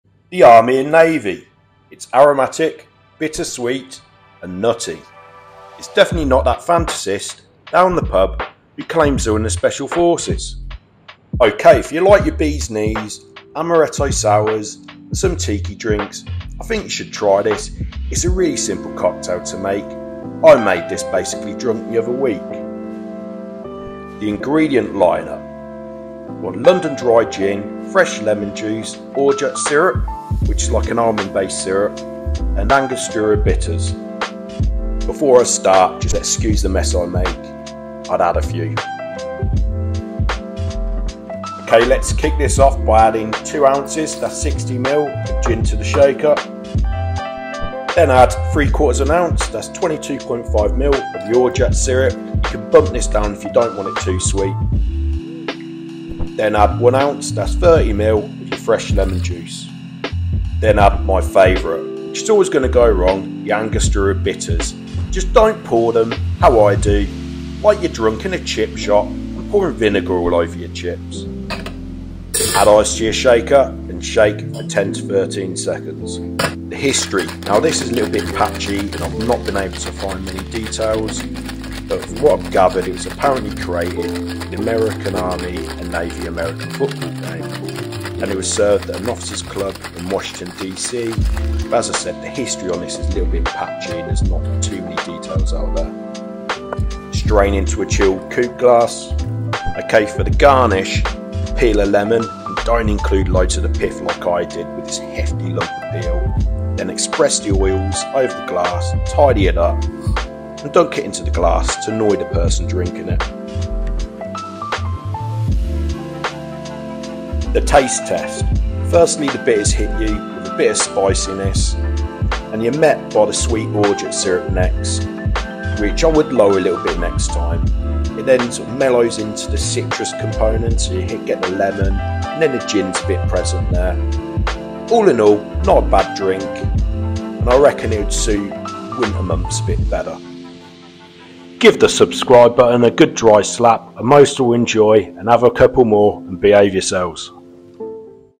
Conquering The Army and Navy Cocktail: A Boozy Mission
Firstly, wherever you are in the world, I hope you're having a great weekend. I made this drink the other weekend, after I was a bit worse for where, so had my own mission.

The Army and Navy is a unique cocktail, has very classical elements with Gin and Citrus, but has some exotic flavours associated with tiki drinks like Orgeat Syrup, and Spicy Bitters.

Ingredients
2 Oz (60ML) London Dry Gin
3/4 Oz (22.5ML) Orgeat Syrup
1 Oz (30ML) Fresh Lemon Juice
1-2 Dashes of Angostura Bitters